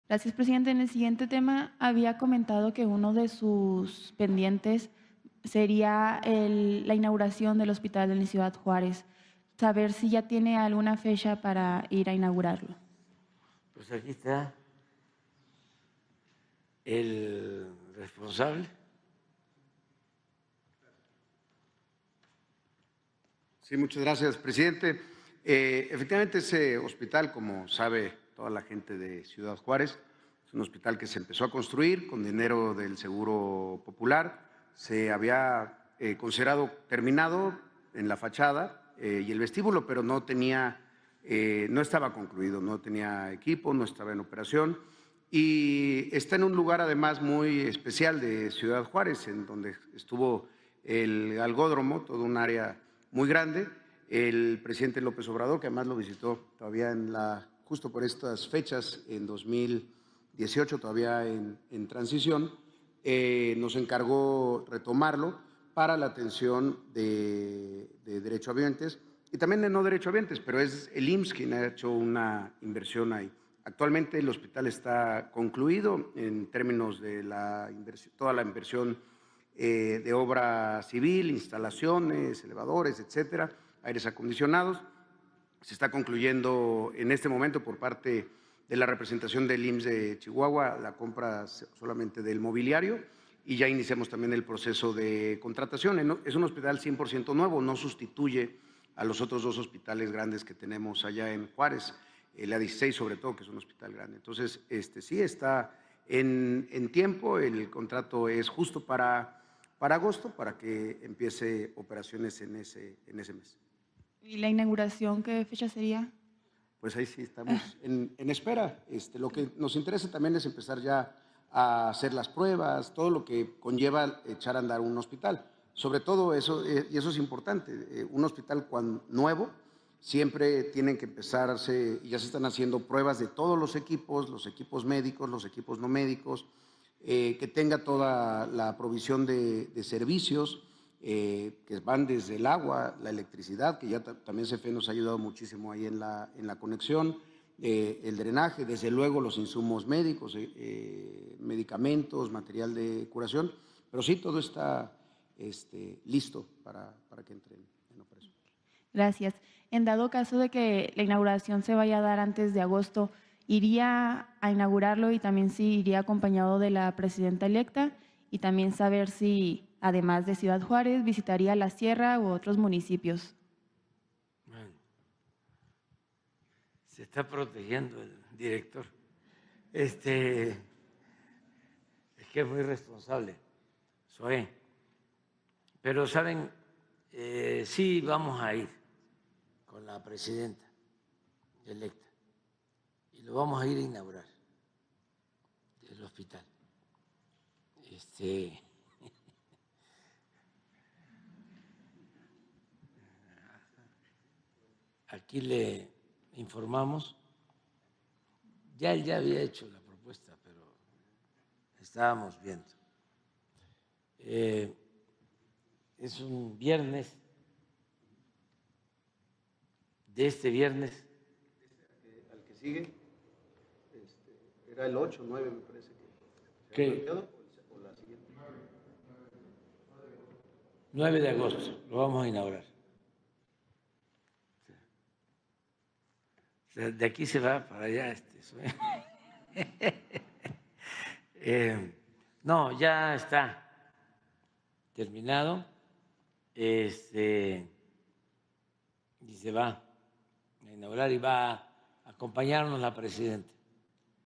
¡LISTO HOSPITAL DE CD. JUÁREZ! AMLO REVELA QUE LO INAUGURARÁ JUNTO A LA DRA. SHEINBAUM
Liliana Piña le echó en cara a López Obrador que uno de sus pendientes sería la inauguración del Hospital en Ciudad Juárez y le cuestionó si ya tiene una fecha. El presidente le dio la palabra al director general del Instituto Mexicano del Seguro Social Zoé Robledo, quien comentó que ese hospital se terminó en la fachada y el vestíbulo, pero hasta ahí, por lo que el mandatario les encargó retomarlo para terminarlo y que efectivamente ya está concluido en cuanto a obra civil, instalaciones, etc. Agregó que se está concluyendo solamente con la compra del mobiliario y que están en los procesos de contratación, por lo que espera que empiece en operaciones en agosto, pero están en espera de que se determine una “inauguración”. Por su parte, el mandatario dijo que va a inaugurar dicho hospital con la presidenta electa, la Dra. Claudia Sheinbaum